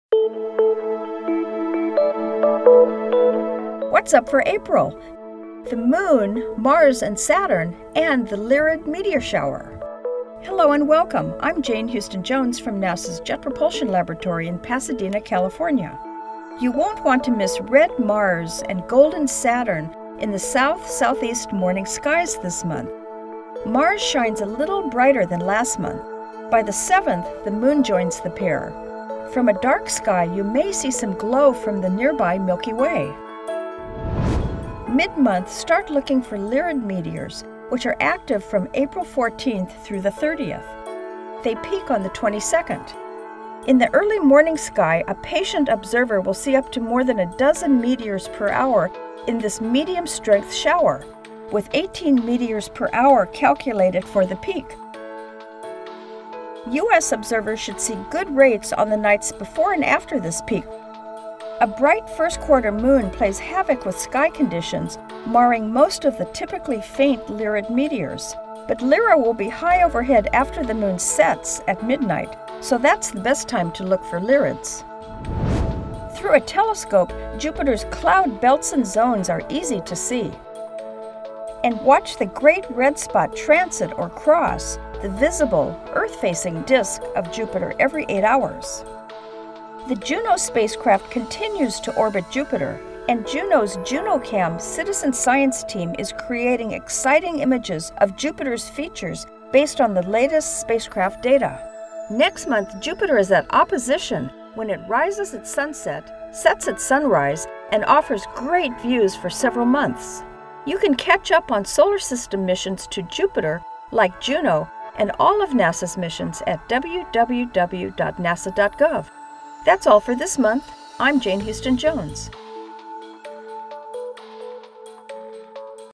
NASA What's Up, April 2018: Mars, Saturn and Lyrids meteor shower
The 2018 edition of the Lyrid meteor shower is under way, and while it's an often underrated and underappreciated event on the annual cosmic calendar, it's worthy of your star gaze. (NASA video)